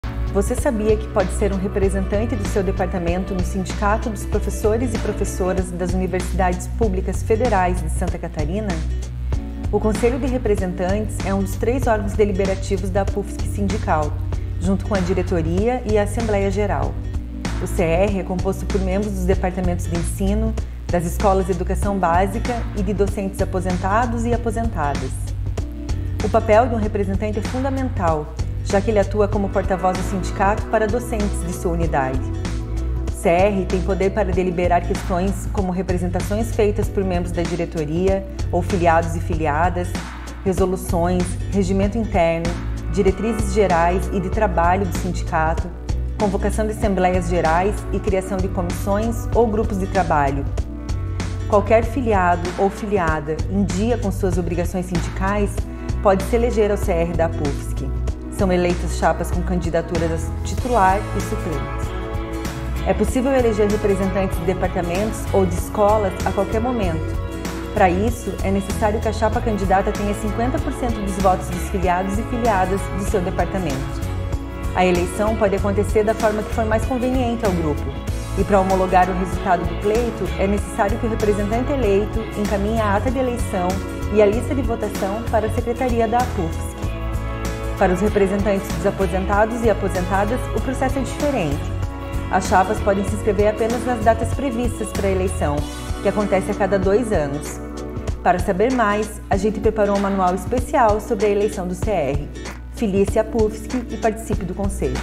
Conselho de Representantes da Apufsc: Saiba como ser porta-voz de seu departamento
O Conselho de Representantes (CR) é um dos três órgãos deliberativos da Apufsc-Sindical, junto com a Diretoria e a Assembleia Geral. O CR é composto por membros dos departamentos de ensino, das escolas de educação básica e de docentes aposentados e aposentadas das universidades federais de Santa Catarina, que são eleitos na primeira quinzena de setembro dos anos ímpares. Quando há vacância de cargo, pode ser realizada eleição para representantes, mas o mandato se encerra juntamente com a renovação do CR. O papel de um representante é fundamental, já que ele atua como porta-voz do sindicato para os docentes da sua unidade.